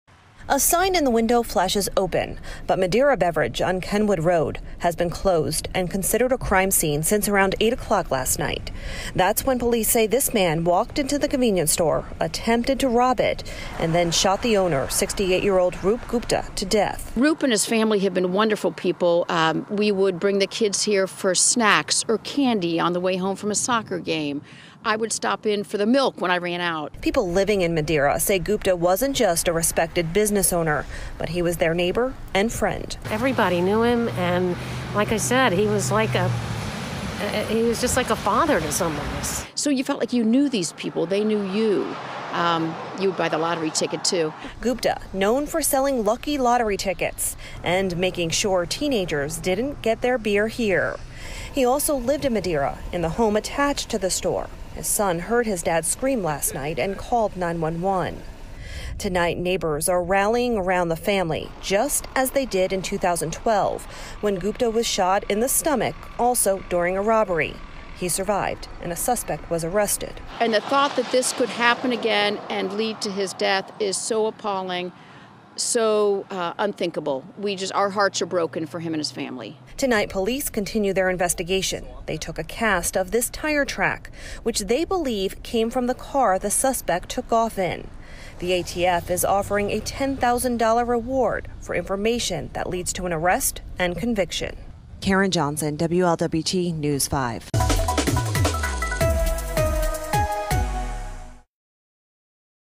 Madeira community rallies around family of slain convenience store owner
A sign in the window of Madeira Beverage flashed 'open' Wednesday, but the convenience store on Kenwood Road has been closed and considered a crime since around 8 p.m. Tuesday.